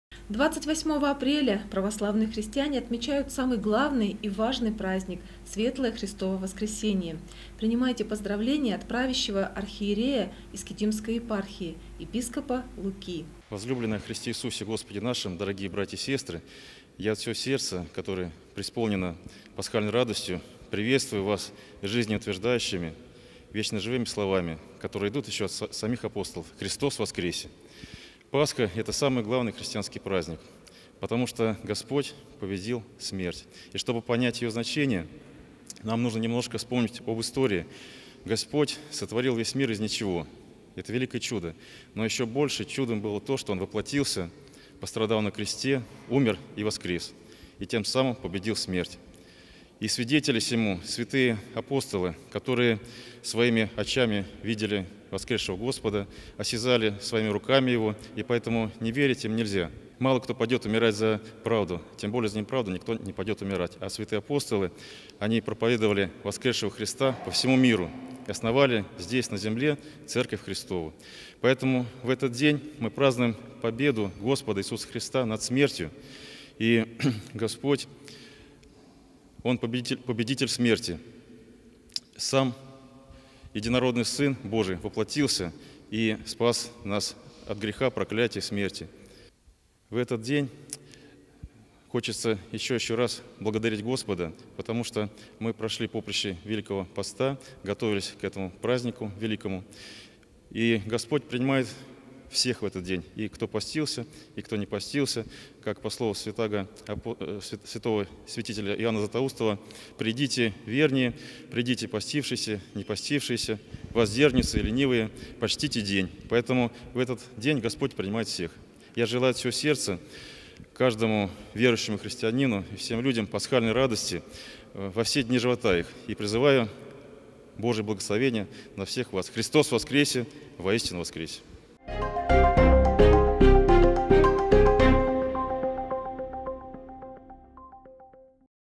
Поздравление с Пасхой
Новости Искитима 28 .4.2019 г.
28 апреля Светлое Христово воскресение.